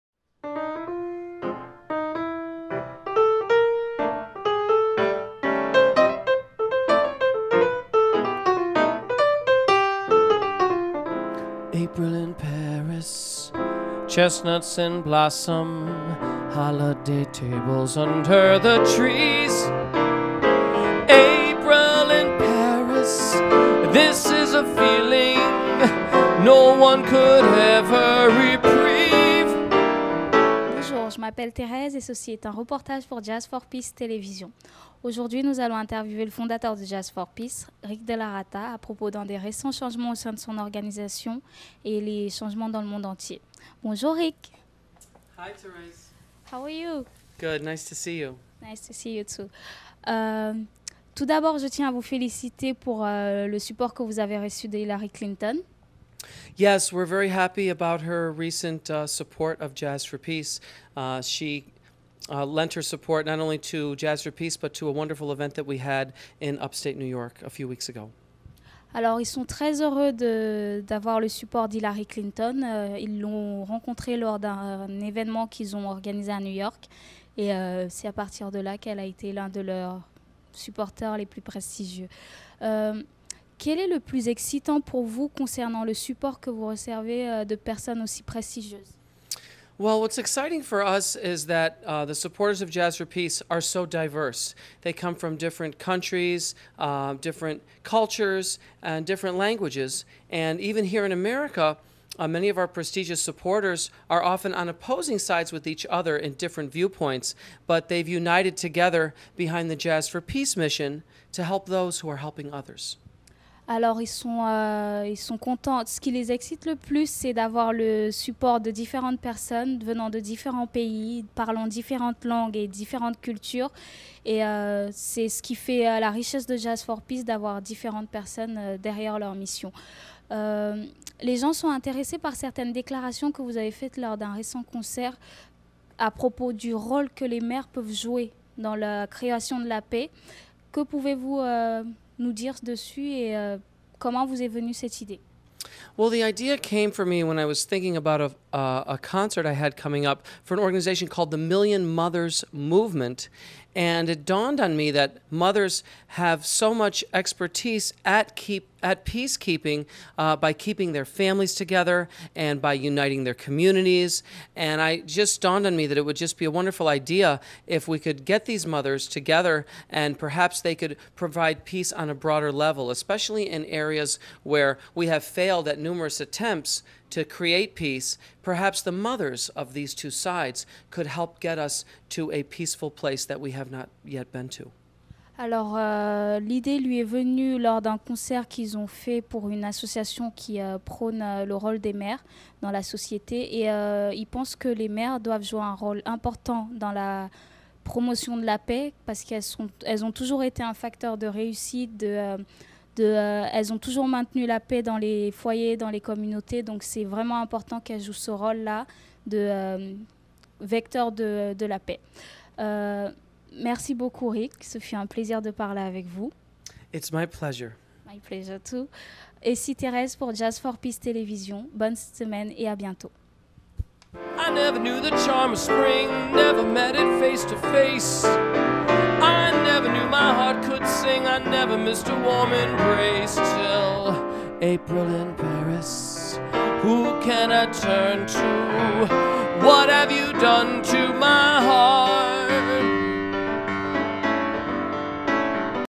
Rick DellaRatta interviewed by Therese from France
Rick DellaRatta interview on Jazz for Peace tv